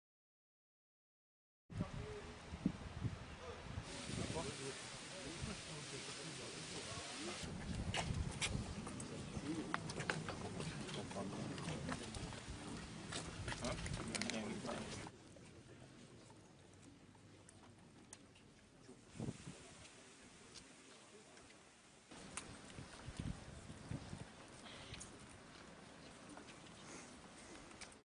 Corse: obsèques de l'avocat assassiné Antoine Sollacaro
Quelque 15O avocats en robe noire, venus de toute la France, ont rendu vendredi 19 octobre un dernier hommage à leur confrère assassiné Antoine Sollacaro, au cours de ses obsèques dans son village corse de Propriano auxquelles ont assisté un millier de personnes. Images. Durée: 00:28.